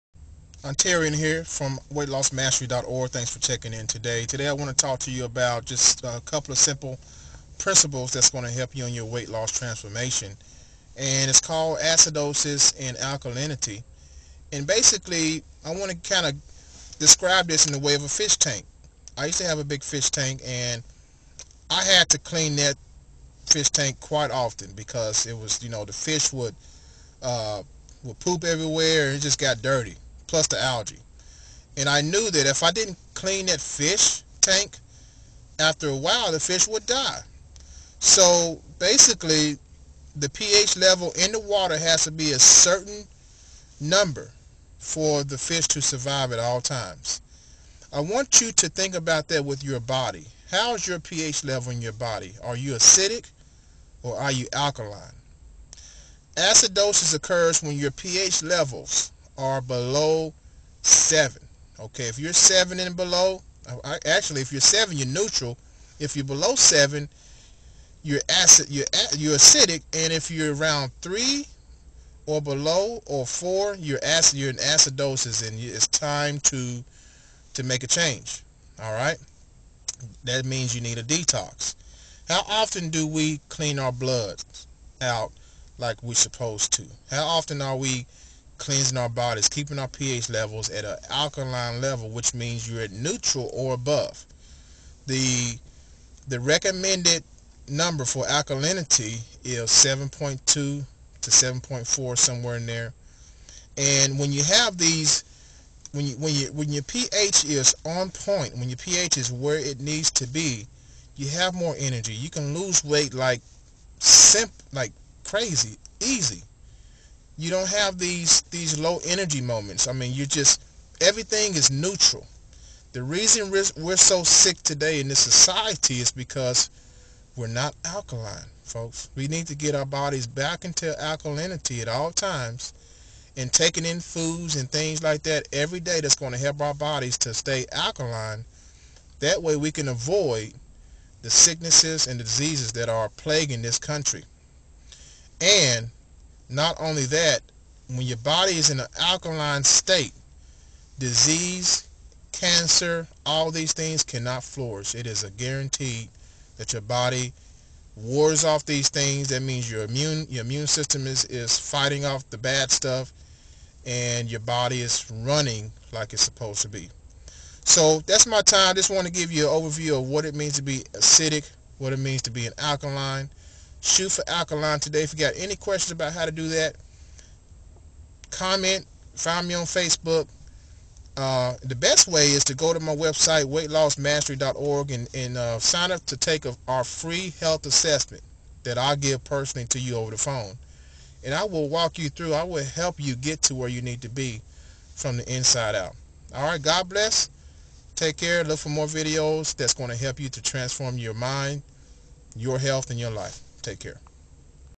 How to Start Losing Weight Naturally and Fast- Alkalinity vs. Acidosis
How to Start Losing Weight Naturally and Fast- Alkalinity vs Acidosis. Are you finally ready to start losing weight quick and fast. Well, this video is going to give you an insight on what's the most important move for you to make right now, which is getting your body into an alkaline state. The more acidic your ph blood level, the more difficult it will be to lose weight.

There are natural ways of cleaning your blood that I will explain in future videos, however, in this video I want you to grasp the concept of what it means to be in either state. If you feel like you don't have much energy, suffer from lack of sleep, have cravings, etc.. Then these are prime indicators that your body is very acidic. Once you reach an alkaline state in your body, you will start to lose weight very fast and efficiently.